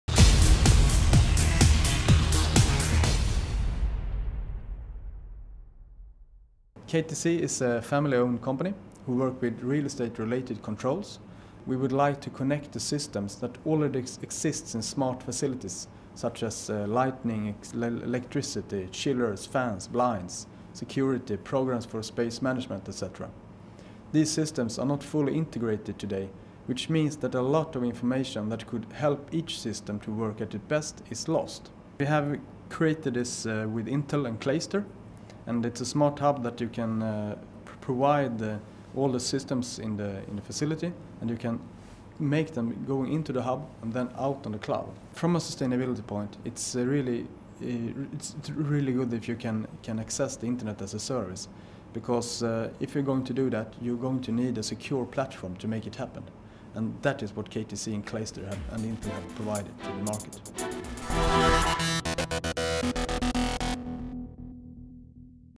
Connecting buildings securely to the cloud
Originally Published on TelecomTV.com 11 Nov 2015

Introduction
KTC Control would like to connect the systems that already exist in buildings - lighting, chillers, fans, blinds, security, space managment. At present they’re not fully integrated but KTC have created a hub so that information can go out to the cloud and the systems can work together.
Filmed at: Intel IoT Ignition Lab, Kista, Sweden, 17 March, 2015
Related ResourcesTwitter
Follow Company @ktc_ab